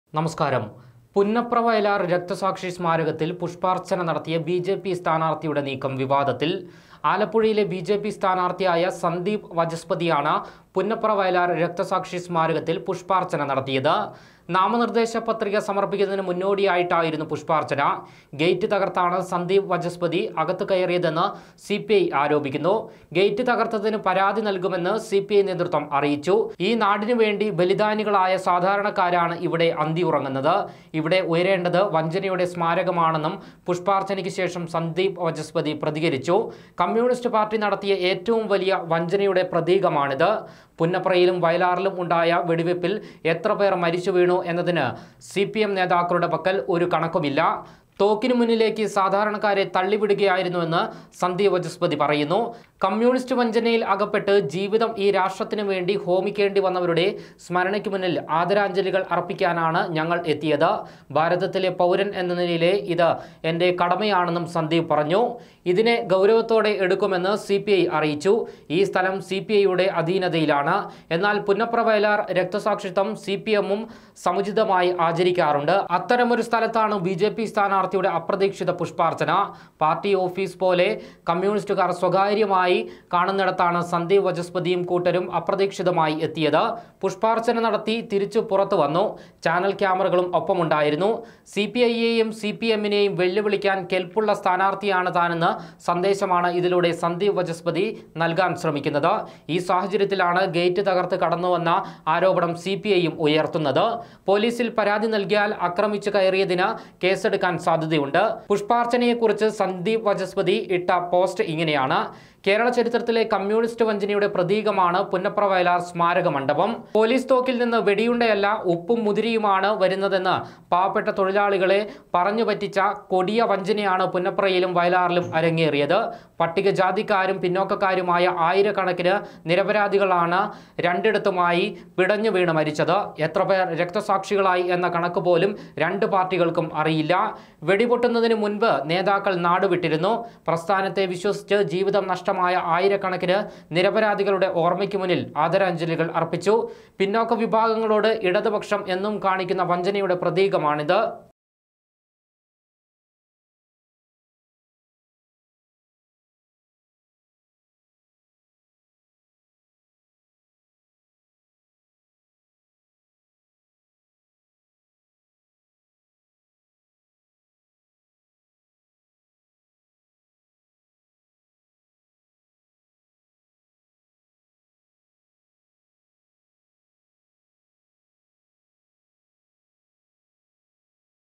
പുന്നപ്ര വയലാർ രക്ഷസാക്ഷി സ്മാരകത്തിൽ പുഷ്പാർച്ചന നടത്തി സന്ദീപ് വചസ്പതി l sandeep vachspathi
ഈ നാടിന് വേണ്ടി ബലിദാനികളായ സാധാരണക്കാരാണ് ഇവിടെ അന്തിയുറങ്ങുന്നത്; ഇവിടെ ഉയരേണ്ടത് വഞ്ചനയുടെ സ്മാരകം; പുന്നപ്ര വയലാർ രക്ഷസാക്ഷി സ്മാരകത്തിൽ പുഷ്പാർച്ചന നടത്തി സന്ദീപ് വചസ്പതി; രക്തസാക്ഷി മണ്ഡപത്തിന്റെ ഗേറ്റ് തകർത്ത് ബിജെപി സ്ഥാനാർത്ഥി അകത്തു കയറിയെന്ന് സിപിഐ; ആലപ്പുഴയിൽ ആരും സ്വപ്‌നം പോലും ഇതുവരെ കണ്ടിട്ടില്ലാത്ത വിവാദം